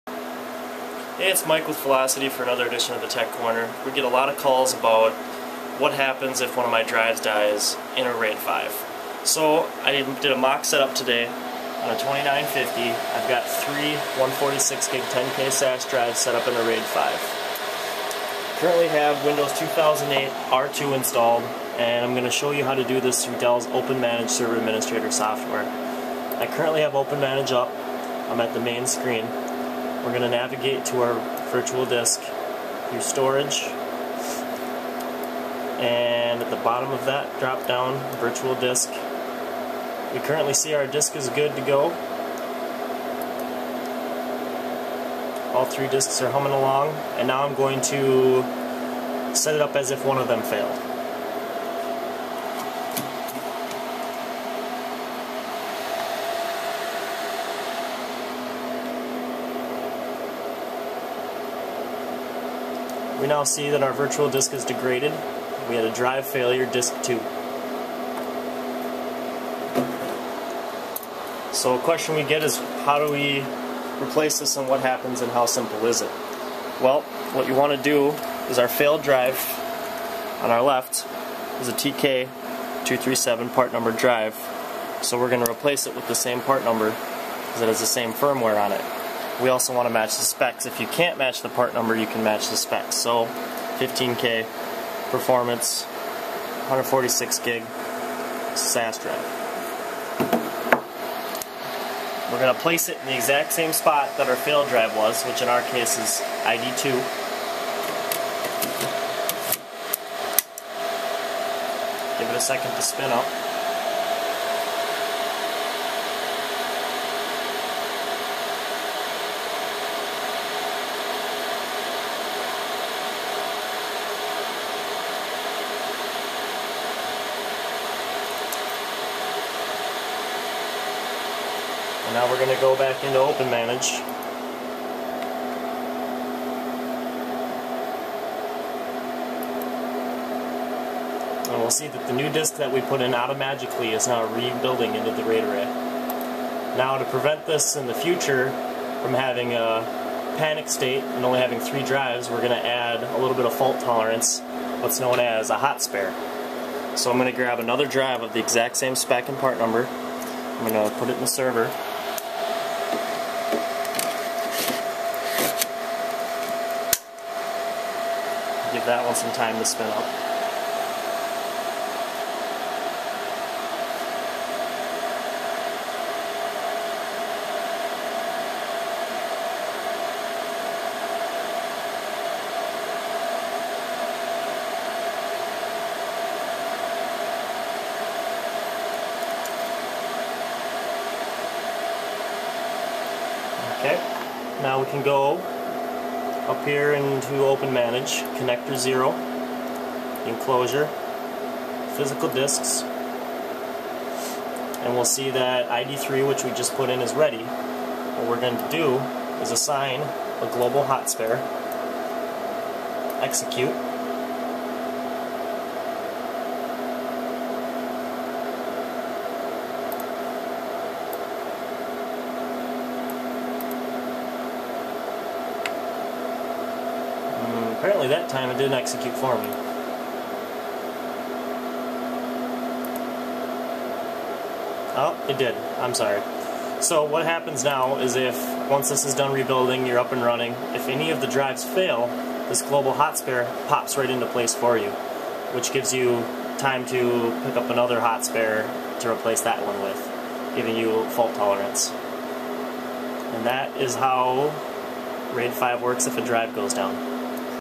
Velocity Tech Solutions - How to Replace or Add a Hard Drive to PowerEdge 2950 Perc 5 Raid 5
Velocity Tech Solutions presents "How to Replace or Add a Hard Drive to a PowerEdge 2950 Perc 5 Raid 5" as a step-by-step tutorial.